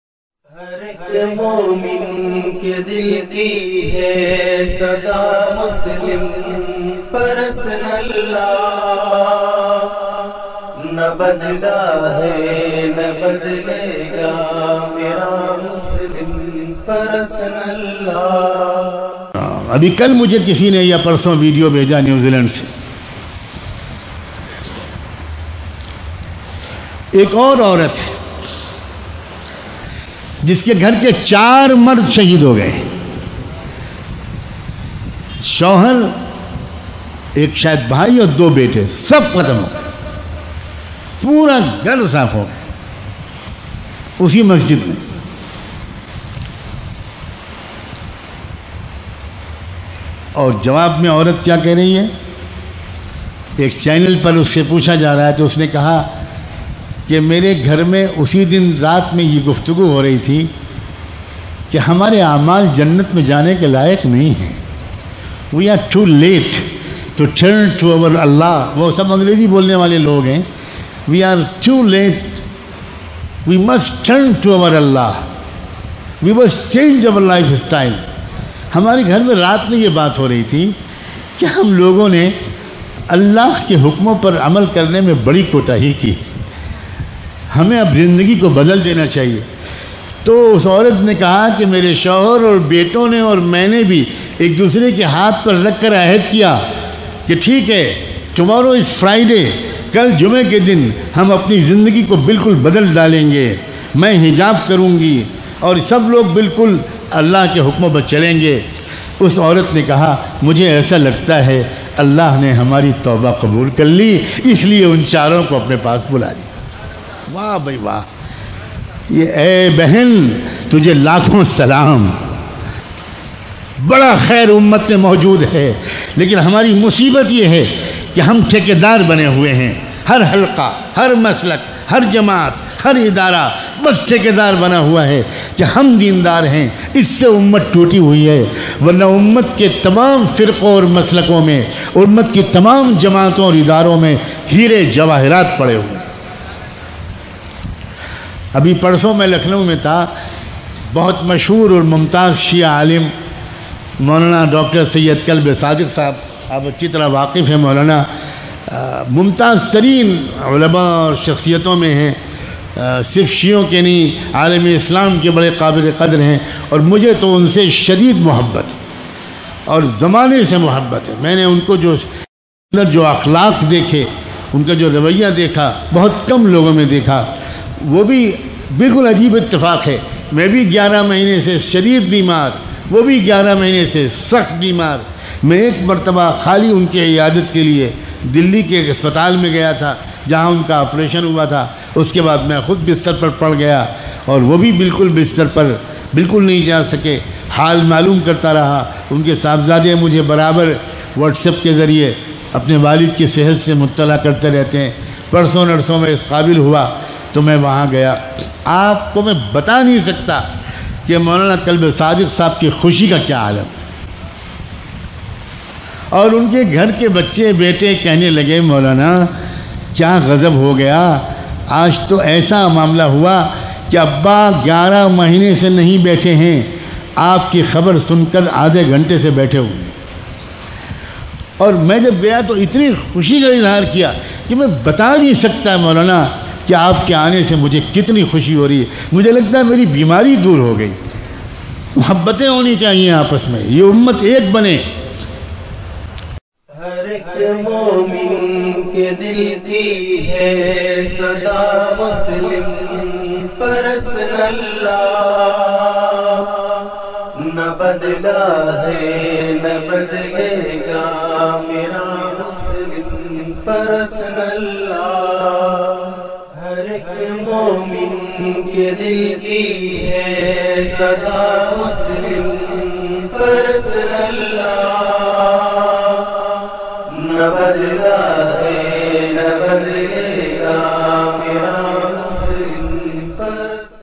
हमें अपनी ज़िन्दगी इस्लामी बनाना चाहिए || Maulana Khalil Ur Rahman Sajjad Nomani DB
हमें अपनी ज़िन्दगी इस्लामी बनाना चाहिए  ||
Hame Apni Zindagi Islami Banana Chahiye -

Maulana Khalil Ur Rahman Sajjad Nomani DB
((Member All India Muslim Personal Law Bard)